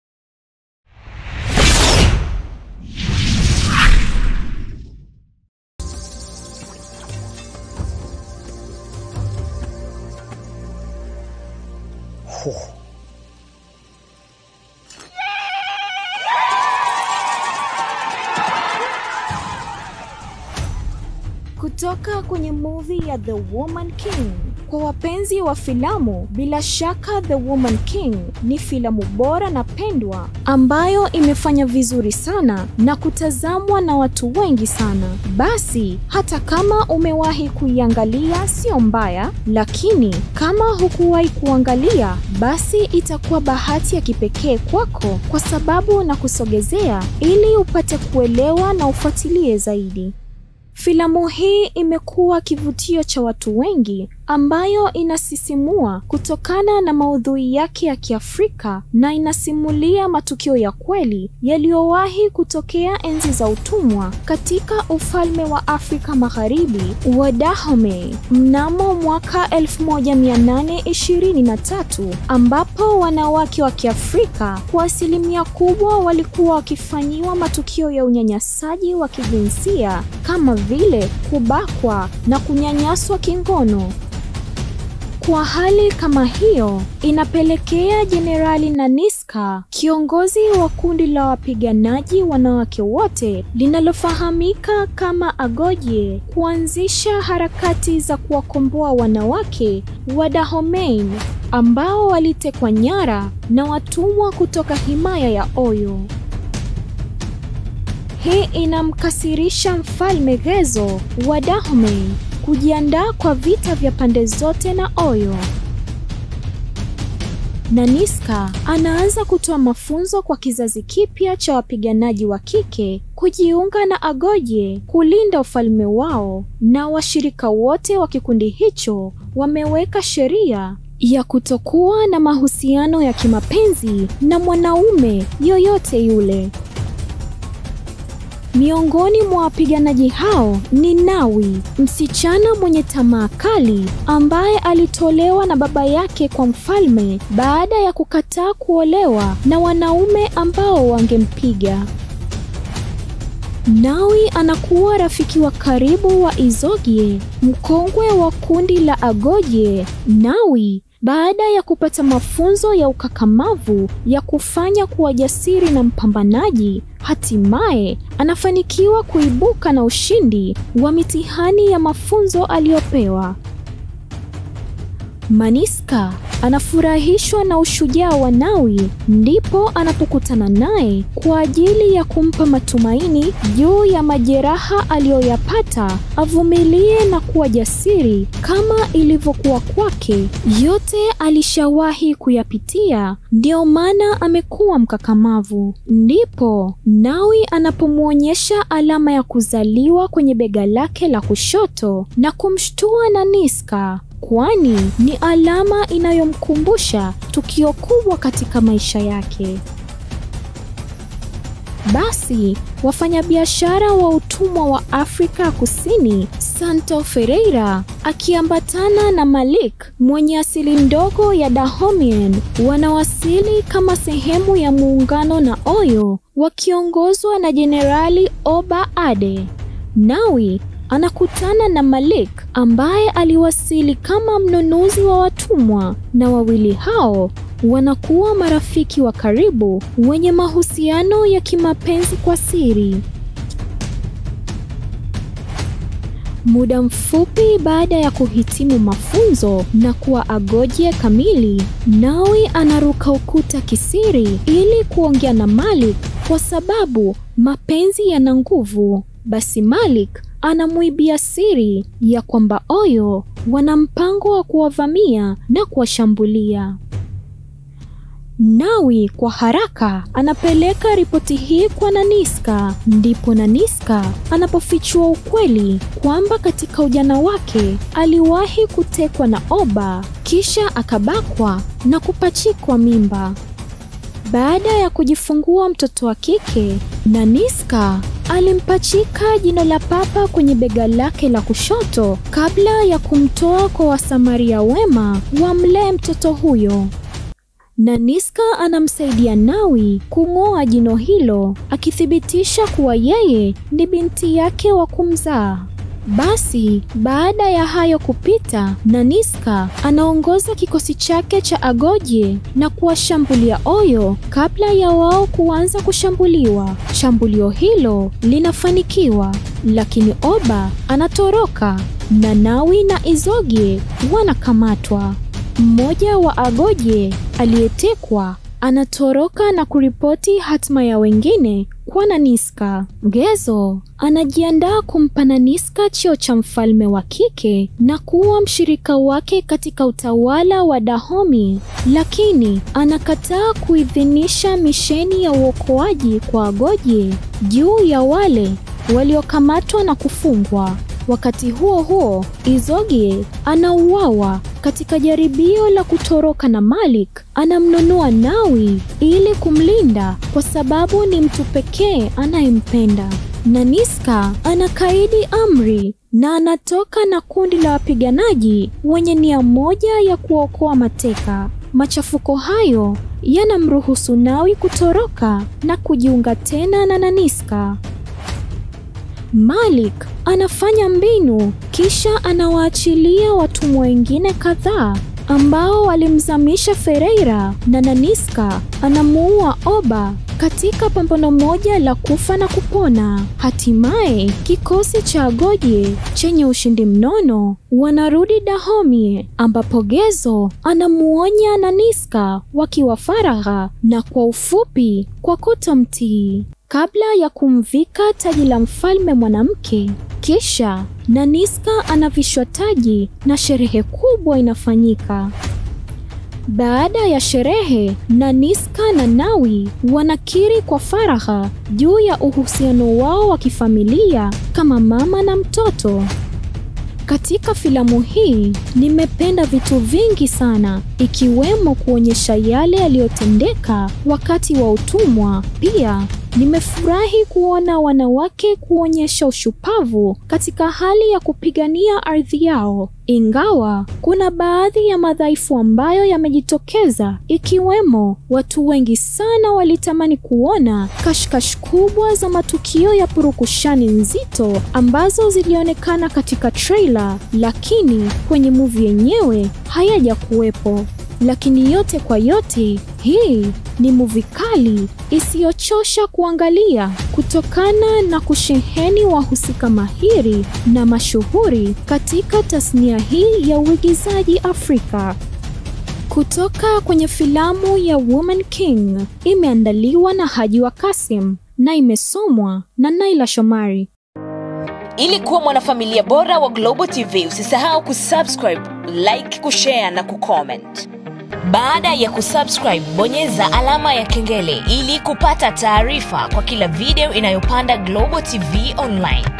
THE WOMAN KING: JESHI HATARI la WANAWAKE | HD Movie | Review in SWAHILI...
KWENYE FILM REVIEWS ya wiki hii tumekusogezea filamu mpya ya mwaka 2022, Woman King...